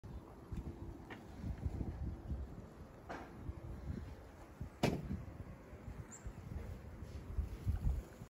Flyby my speckled peas a week after seeding
My speckled peas seem to be ready for harvest about 10 days after seeding. I think I’ll seed every 2 weeks to give myself a couple of days to harvest, package and deliver. Happy seeding!